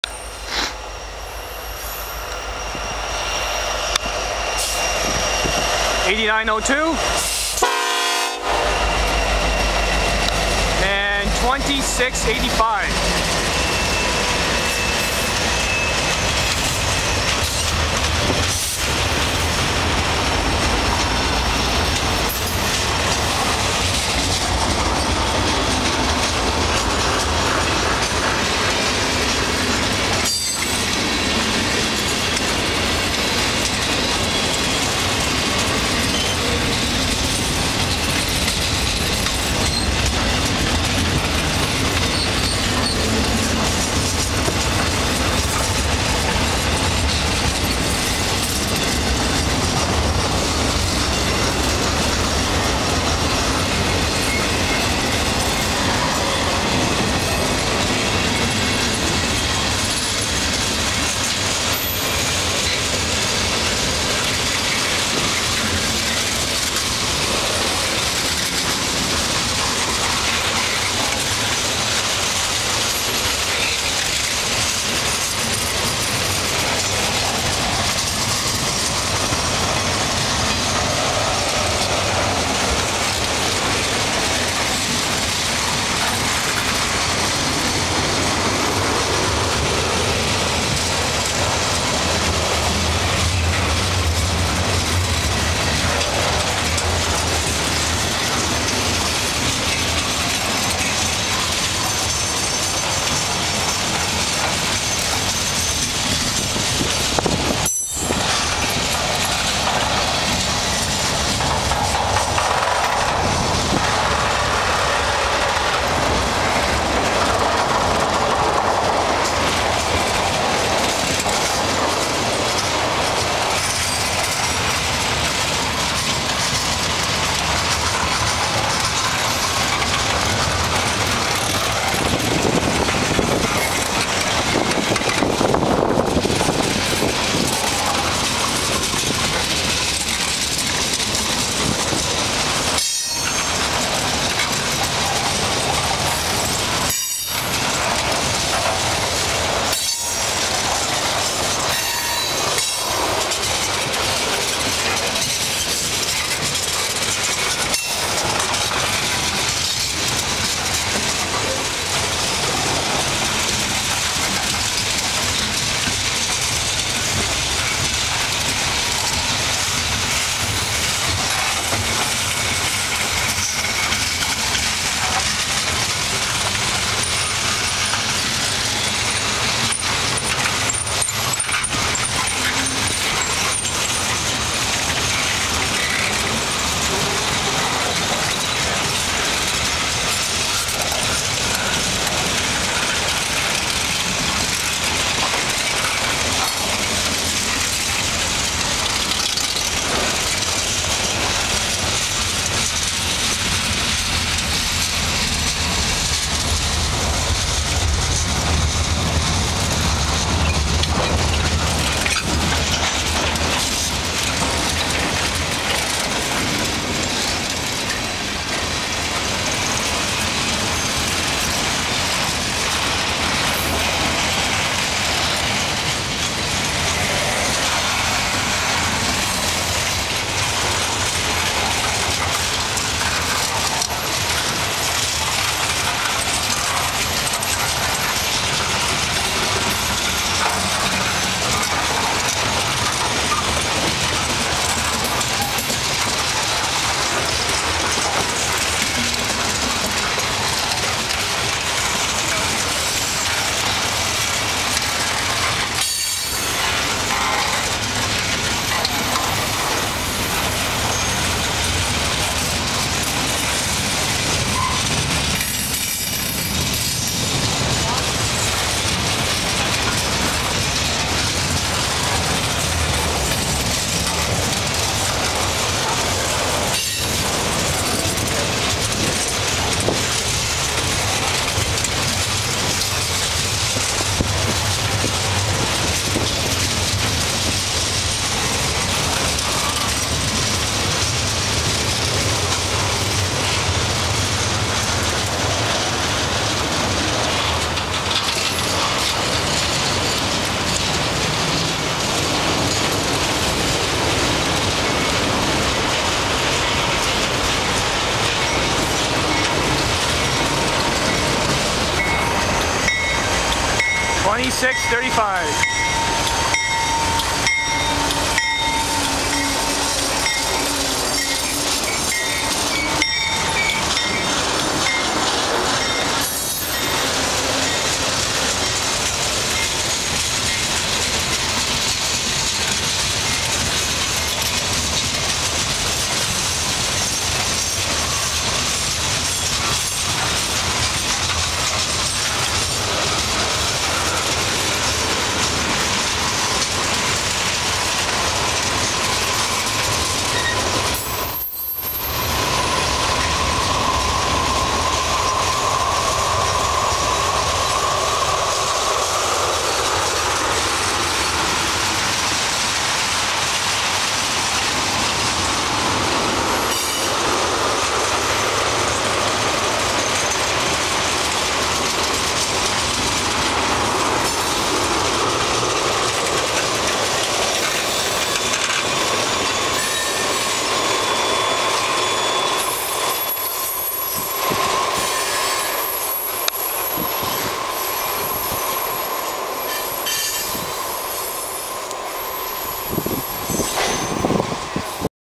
Upload #600 ! w/CN Engine's 8902 & 2685 lead this long Autoracker/mixed freight train.
thru Bayview in Hamilton.

heres my 4th catch from today and my 600th upload ! with horn action which scared the SH** outta me ! haha

Jay (crazeejay2006), Nick (widecabfan) Mike (‪TheEtobicokeRailfan), Dan (‪dan9325), StCatharinesRailFan8, OntarioRails1 & Ryan (Localrailfan) were all there.

video taken- Saturday January 22th 2011 at 11:03am
and as always, Comment & Enjoy !
thnxs ! =]